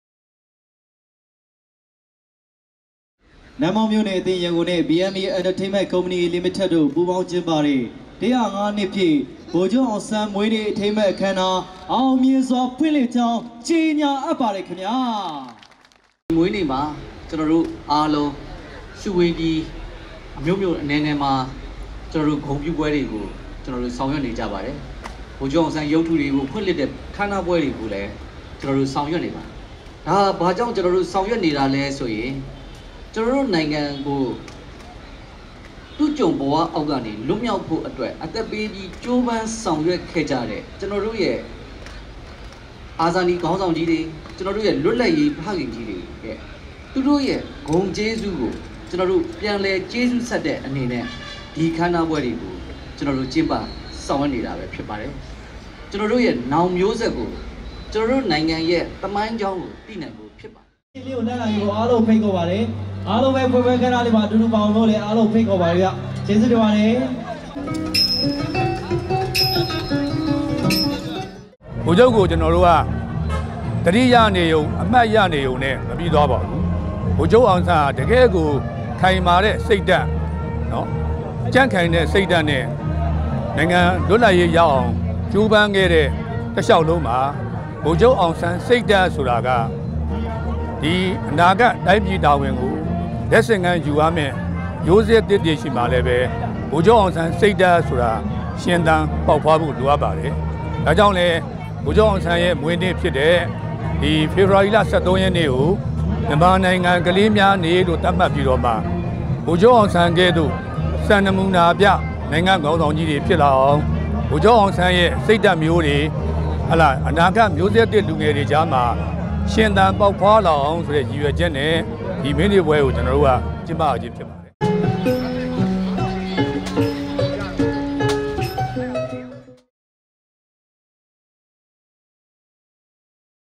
ဗိုလ်ချူပ်အောင်ဆန်း (၁၀၅)နှစ်ပြည့် မွေးနေ့အထိမ်းအမှတ်အခမ်းအနားပွဲ ကျင်းပ
ဗိုလ်ချူပ်အောင်ဆန်း (၁၀၅)နှစ်ပြည့် မွေးနေ့အထိမ်းအမှတ်အခမ်းအနားပွဲ ကျင်းပ (ရုပ်/သံ)

ဧရာဝတီ၊ ဖေဖော်၀ါရီ ၁၃၊ ၂၀၂၀

နတ်မောက်မြို့နယ်အသင်း(ရန်ကုန်)နှင့် BMB Entertainment Co.,Ltd တို့ ပူးပေါင်းပြီး ဗိုလ်ချုပ်အောင်ဆန်း(၁၀၅)နှစ်ပြည့် မွေးနေ့အထိမ်းအမှတ်အခမ်းအနားကို ရန်ကုန်မြို့ မဟာဗန္ဓုလပန်းခြံရှေ့မှာ ကျင်းပပြုလုပ်ခဲ့သည်။

ရိုက်ကူး/တည်းဖြတ် − စည်သူ